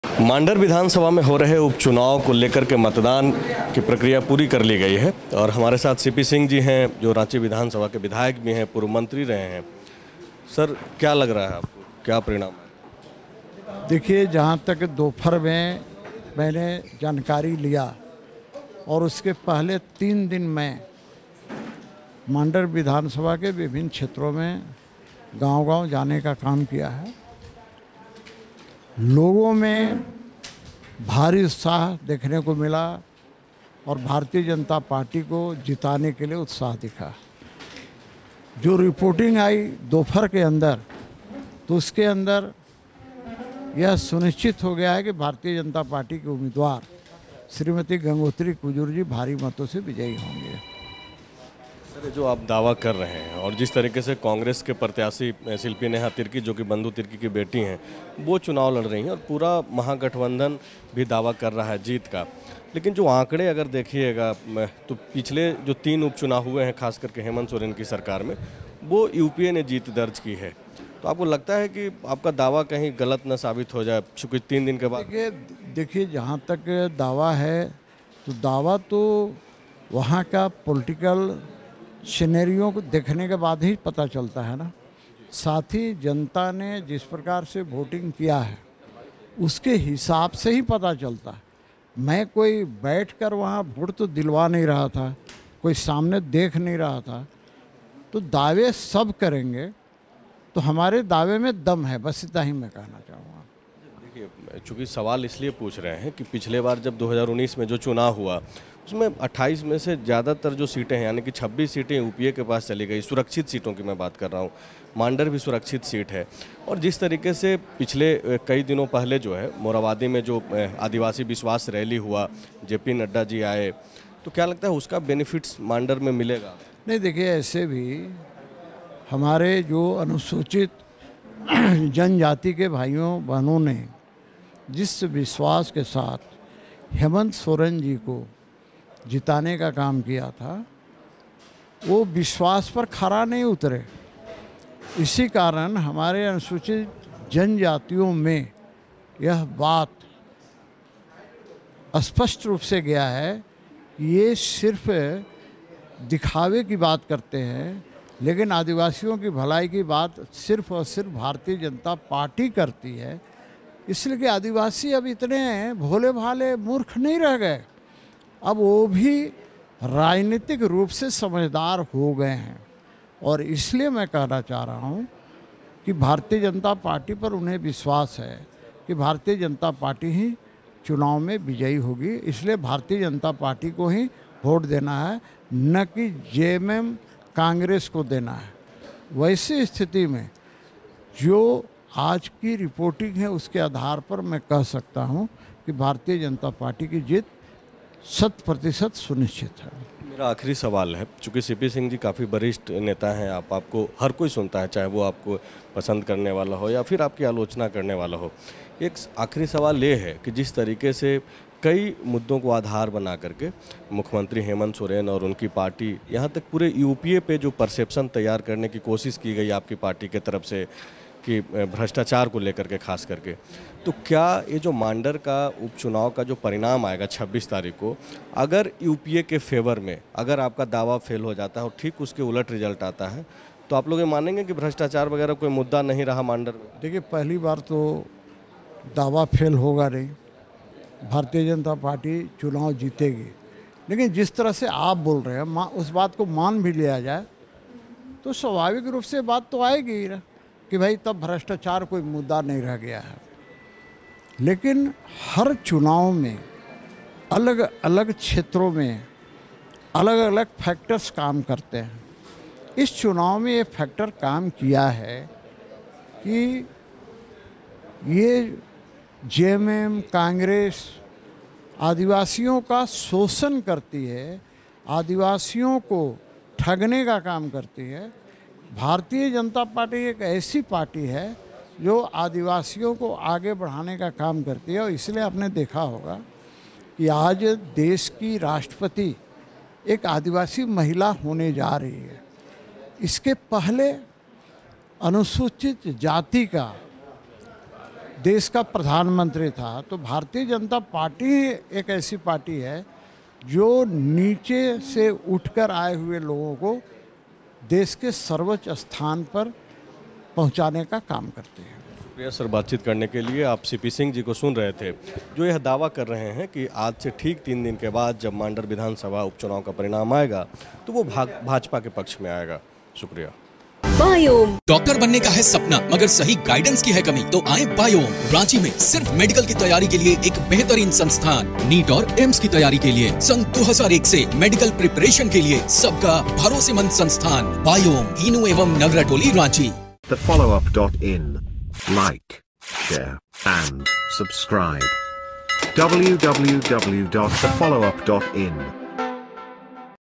CP Singh ने इस आधार पर Mandar में जीत का किया दावा, कहा- सौ फीसदी निश्चिंत हैं, जीतेंगे
सीपी सिंह ने इस आधार पर मांडर में जीत का किया दावा, कहा- सौ फीसदी निश्चिंत हैं, जीतेंगे